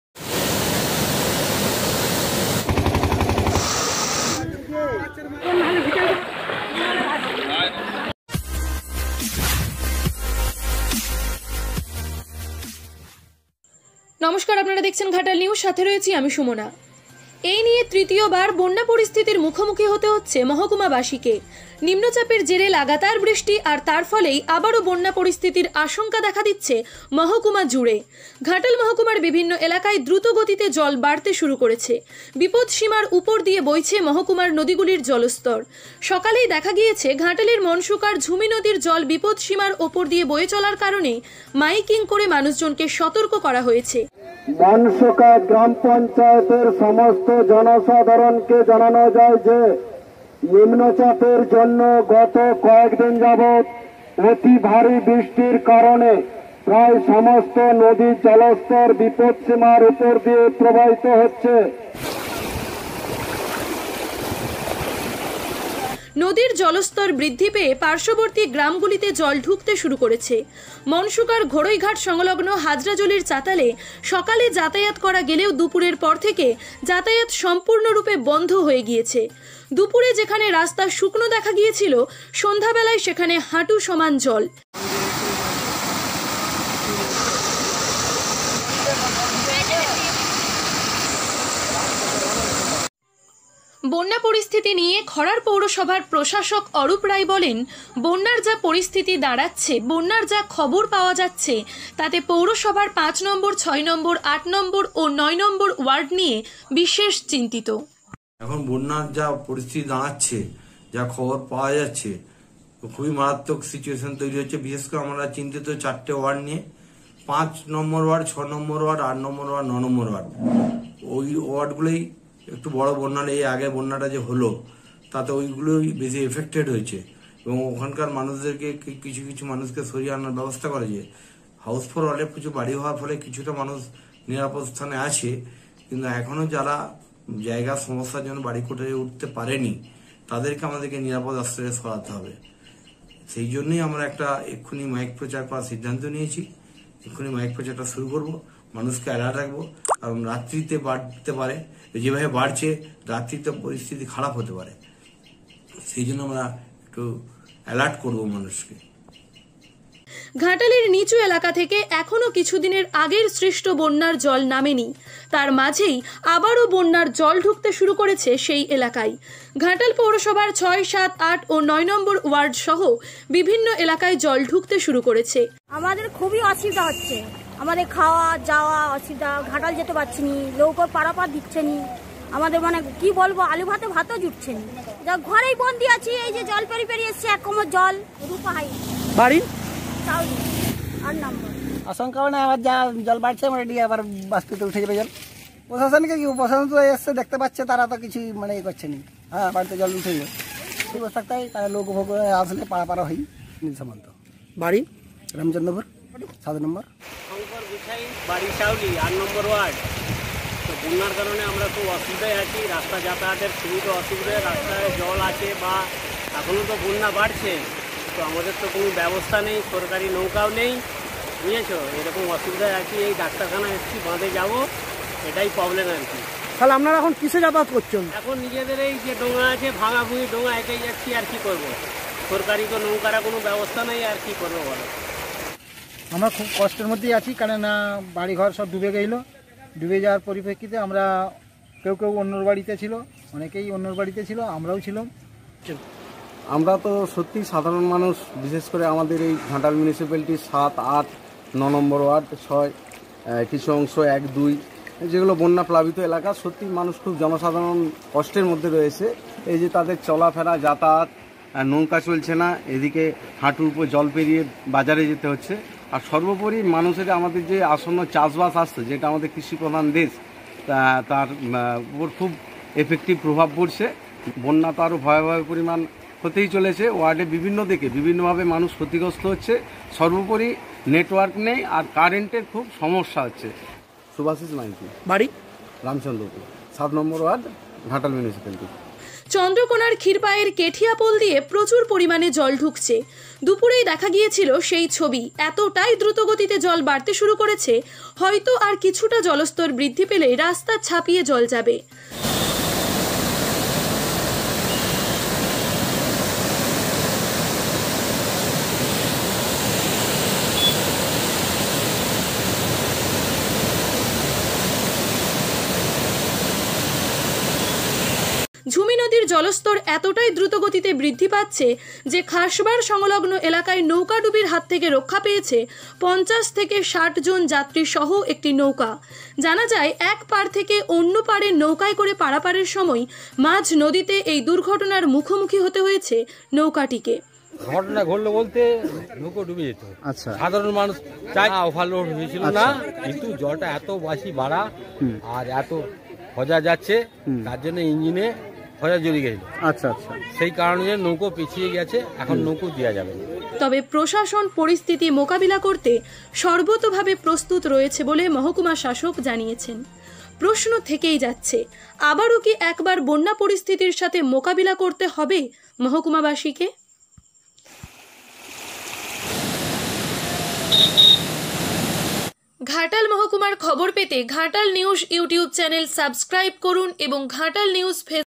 অাবারও ঘাটাল বন্যা পরিস্থিতির মুখোমুখি হতে হচ্ছে
এই নিয়ে চতুর্থবার বন্যা পরিস্থিতির মুখোমুখি হতে হচ্ছে মহাকুমা বাসীকে।
     নিম্নচাপের জেরে লাগাতার বৃষ্টি তার ফলেই আবারও বন্যা পরিস্থিতির আশঙ্কা দেখা দিচ্ছে মহকুমা জুড়ে।
      ঘাটাল মহকুমার বিভিন্ন এলাকায় দ্রুতগতিতে জল বাড়তে শুরু করেছে। বিপদসীমার উপর দিয়ে বইছে মহকুমার নদীগুলির জলস্তর।
      সকালেই দেখা গিয়েছে ঘাটালের মনসুকার ঝুমি নদীর জল বিপদসীমার উপর দিয়ে বয়ে চলার কারণে মাইকিং করে মানুষজনকে সতর্ক করা হয়েছে।নদীর জলস্তর বৃদ্ধি পেয়ে পার্শ্ববর্তী গ্রামগুলিতে জল ঢুকতে শুরু করেছে। মনসুকার ঘোড়ইঘাট সংলগ্ন হাজরাজলির চাতালে সকালে যাতায়াত করা গেলেও দুপুরের পর থেকে যাতায়াত সম্পূর্ণরূপে বন্ধ হয়ে গিয়েছে।
      দুপুরে যেখানে রাস্তা শুকনো দেখা গিয়েছিল সন্ধ্যাবেলায় সেখানে হাঁটুসমান জল।
      ঘাটালের নিচু এলাকা থেকে এখনো কিছুদিনের আগের সৃষ্ট বন্যার জল নামেনি তার মাঝেই আবারো বন্যার জল ঢুকতে শুরু করেছে সেই এলাকায়।
       ঘাটাল পৌরসভার ৬,৭,৮ ও ৯ নং ওয়ার্ড সহ বিভিন্ন এলাকায় জল ঢুকতে শুরু করেছে।
       চন্দ্রকোনার ক্ষীরপাই এর কেঠিয়া পোল দিয়ে প্রচুর পরিমাণে জল ঢুকছে।দুপুরেই দেখা গিয়েছিল সেই ছবি। এতটাই দ্রুত গতিতে জল বাড়তে শুরু করেছে হয়তো আর কিছুটা জলস্তর বৃদ্ধি পেলেই রাস্তা ছাপিয়ে জল যাবে।
      ঝুমি নদীর জলস্তর এতটাই দ্রুতগতিতে বৃদ্ধি পাচ্ছে যে খাসবাড় সংলগ্ন এলাকায় নৌকাডুবির হাত থেকে রক্ষা পেয়েছে ৫০-৬০ জন যাত্রী সহ একটি নৌকা। জানা যায়, এক পাড় থেকে অন্য পাড়ে নৌকায় করে পারাপারের সময় মাঝ নদীতে এই দুর্ঘটনার মুখোমুখি হতে হয়েছে নৌকাটিকে।
        তবে প্রশাসন পরিস্থিতি মোকাবিলা করতে সর্বতোভাবে প্রস্তুত রয়েছে বলে মহকুমা শাসক জানিয়েছেন।
       প্রশ্ন থেকেই যাচ্ছে, আবারও কি একবার বন্যা পরিস্থিতির সাথে মোকাবিলা করতে হবে মহাকুমা বাসিকে?